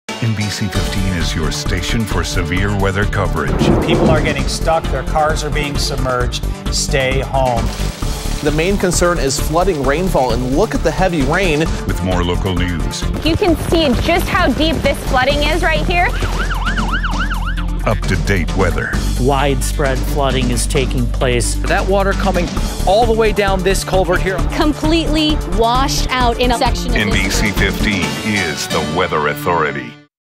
Area Flooding Weather POP
WBA 2018 Weather Promotion Entry
On Monday, Aug. 20 several inches of rain fell in a matter of hours. The water rose so quickly many had to be rescued and countless vehicles towed.